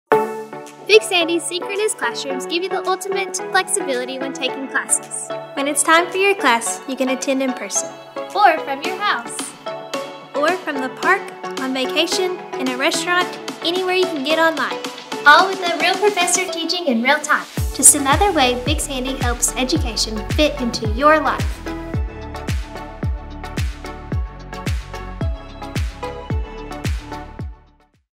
Big Sandy's Synchronous Classrooms
Our Synchronous classrooms allow to choose whether to attend a class remotely or in person using modern technology to make sure that everyone has a chance to participate in the fullest!